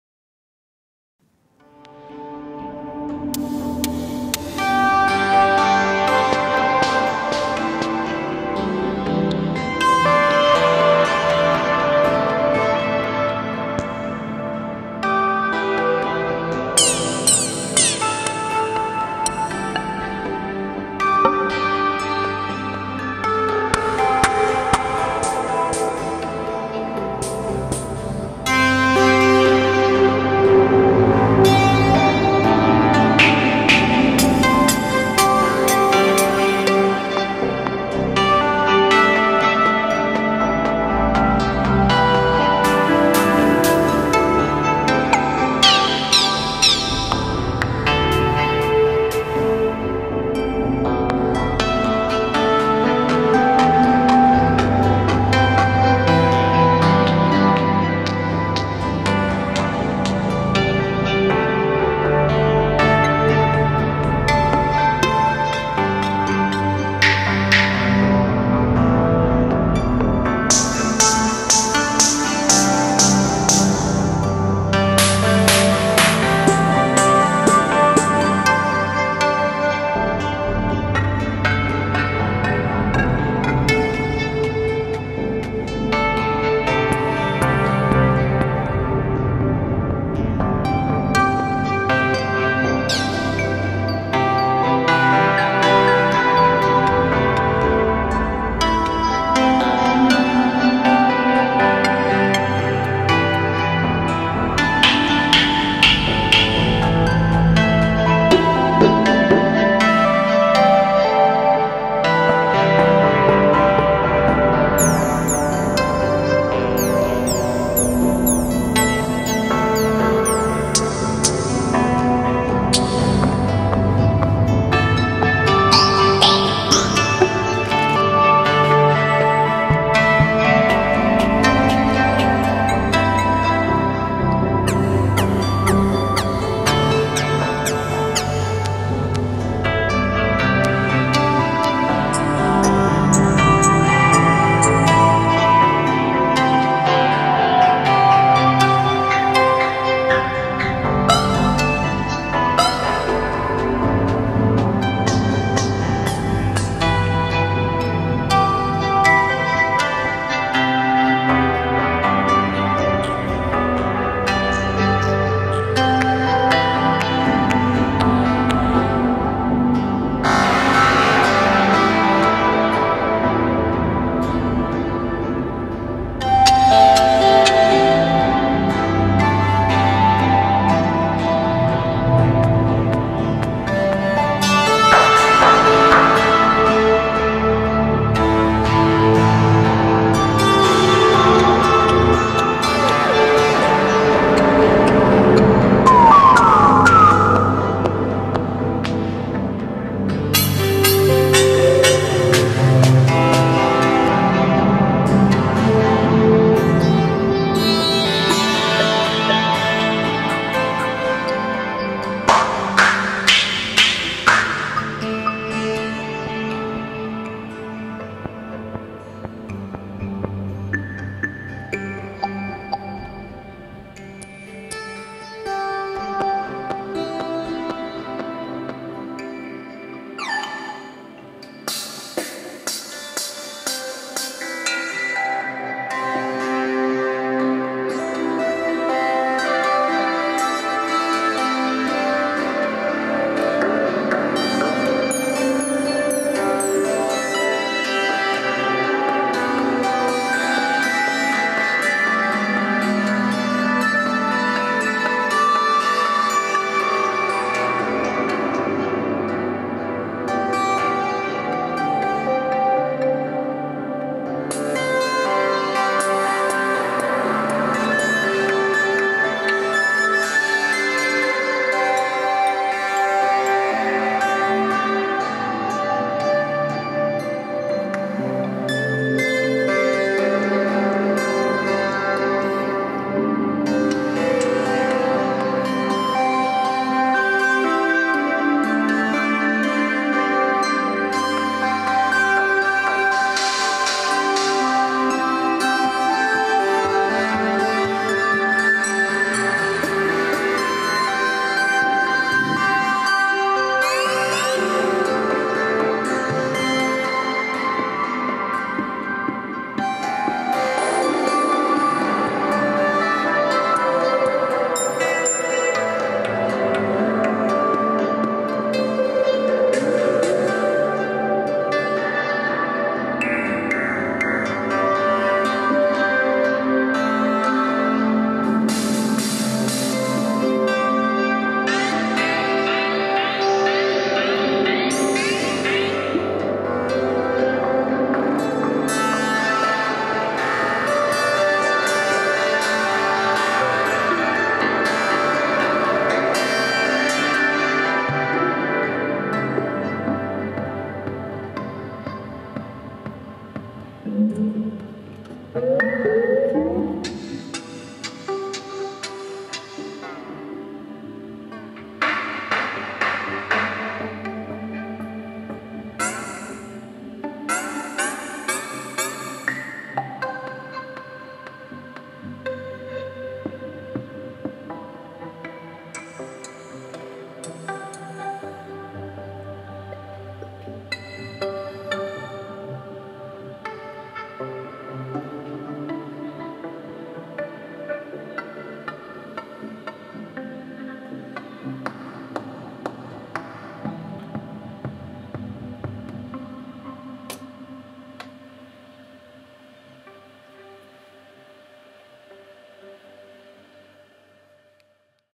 Ambient Generative Modular Jam
Testing out PNW's new firmware. Playing with the new quantizer and learning a bit about the euclidean rhythm settings.